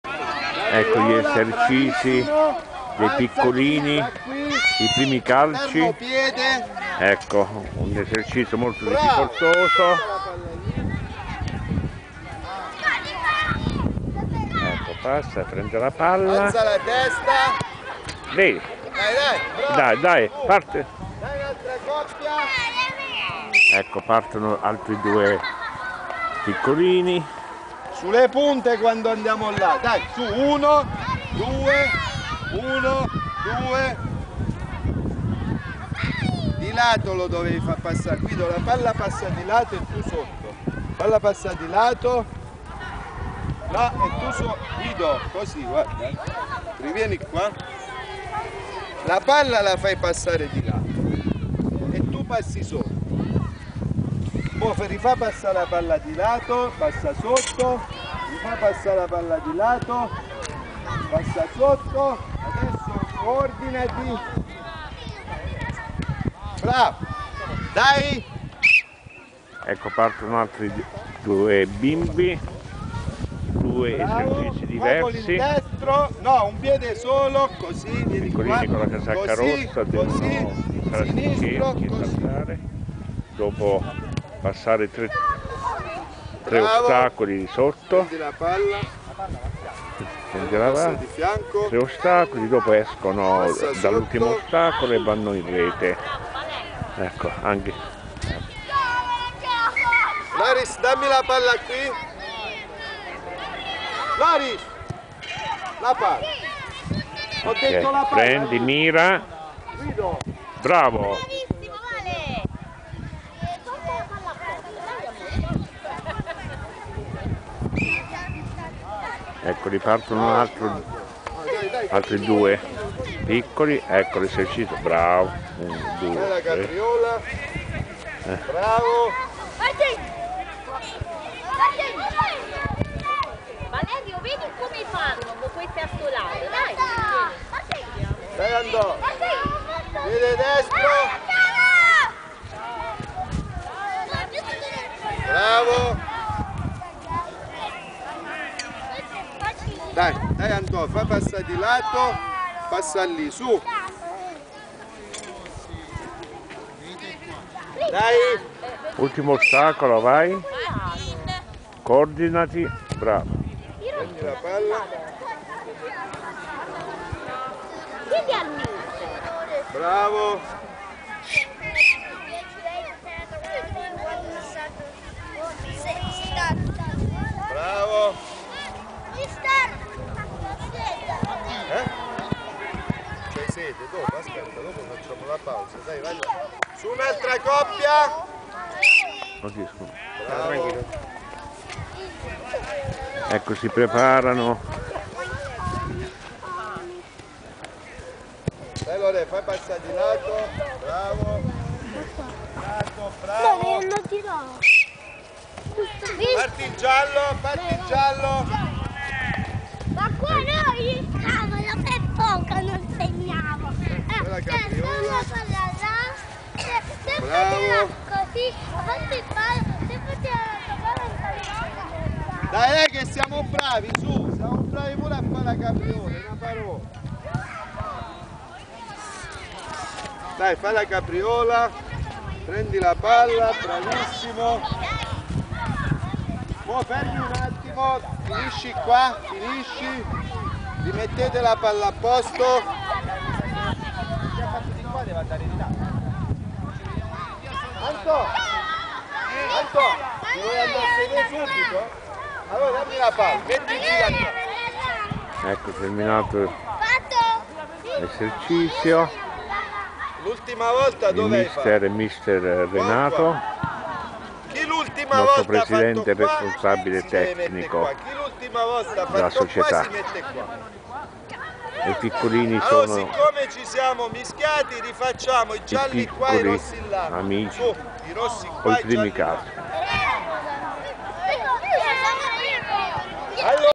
VIRTUS CASTEL FRENTANO SCUOLA CALCIO Stag. 2016 - 2017
La nostra Scuola Calcio in piena attività ,i baby Calciatori ,divisi per categoria si allenano sul manto erboso del " Nuovo " di Via Olimpia sotto lo sguardo vigile del loro Mister Educatore Signor Renato TUCCI nostro Presidente e Responsabile Tecnico